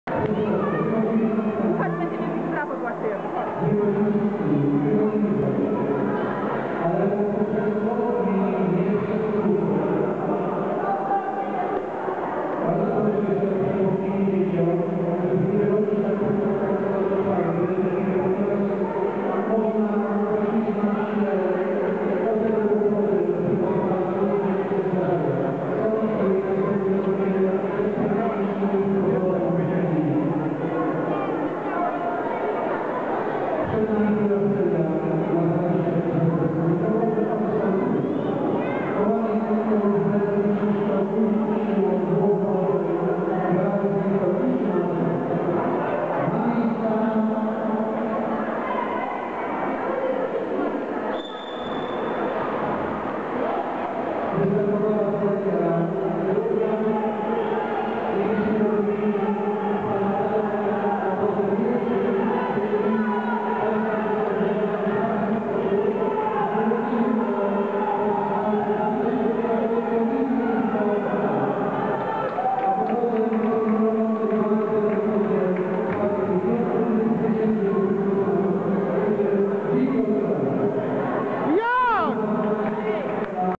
Błażej Błoch - I miejsce dowolnym w zawodach KORONA DZIECIOM
ZAWODY PŁYWACKIE „KORONA -- DZIECIOM"
POD PATRONATEM PREZESA K.S. KORONA KRAKÓW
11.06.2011